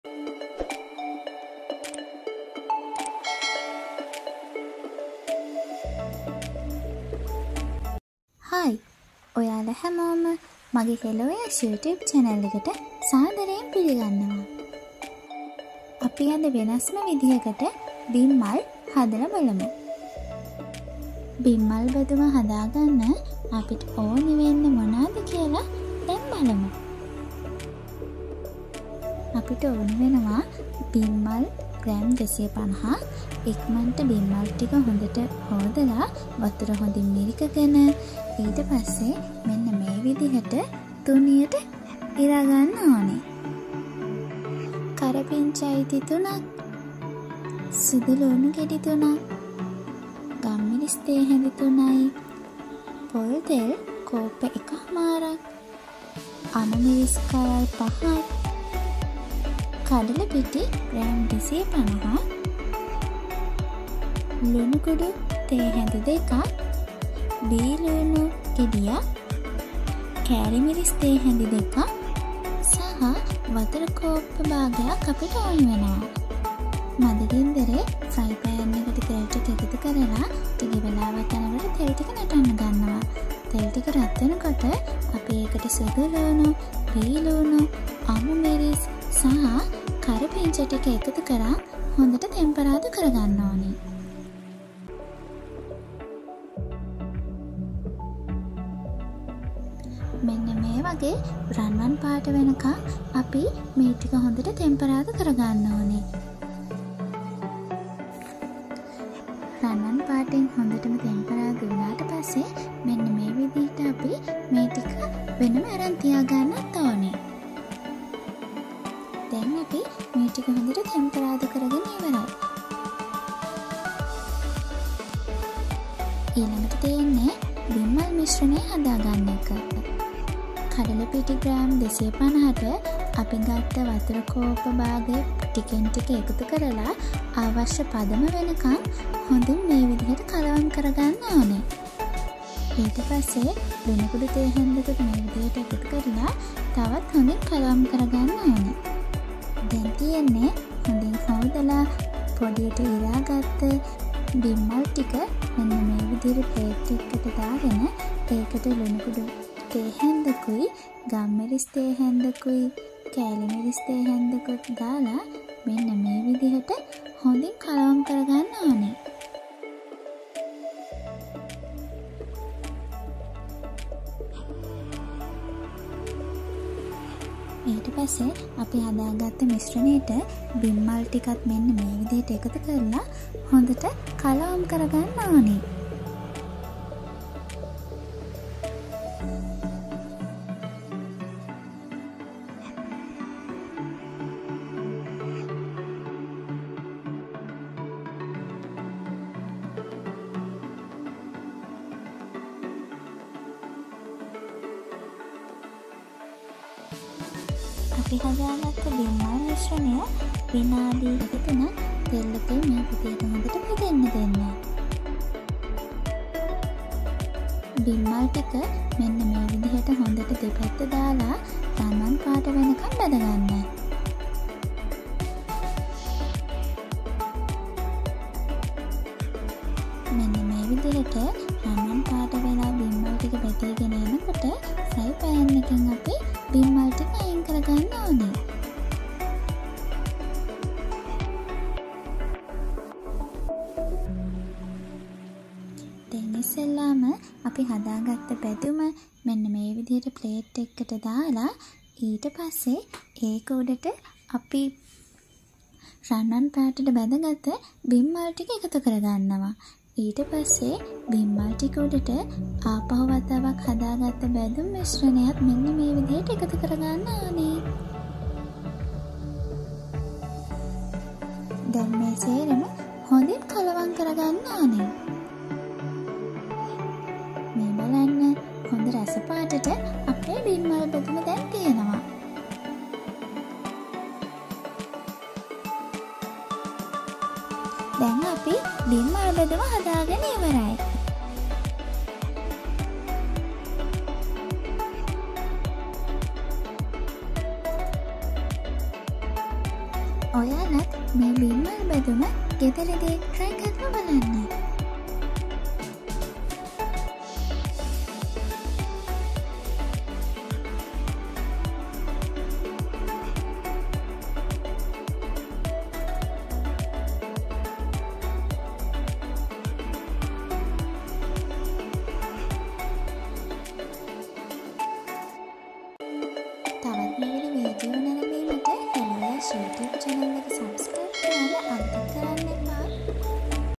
බිම්මල් බැදුමක් හදමු | Let’s make Fried Mushroom | How to make Fried Mushroom | Hello Yash
Ingredients | අවශ්‍ය අමුදව්‍ය

1. 250g of Mushroom -බිම්මල් 250g
2.  Curry Leaves - කරපිංචා ඉති 3
3. Garlic - සුදු ලූණු ගෙඩි 5
4. Pepper - ගම්මිරිස් තේ හැදි 3
5. 1 ½ cup of coconut – පොල්තෙල් කෝප්ප 1 ½
6. Green chilli – අමු මිරිස් කරල් 5
7. 250g of Gram Flour - කඩල පිටි 250g
8. 2 tea spoon of salt - ලුණු කුඩු තේ හැදි 2
9. B Onion - බී ලූණු
10. 2 tea spoon of chilli pieces - මිරිස් කුඩු තේ හැදි 2
11. ½ cup of water - වතුර කෝප්ප ½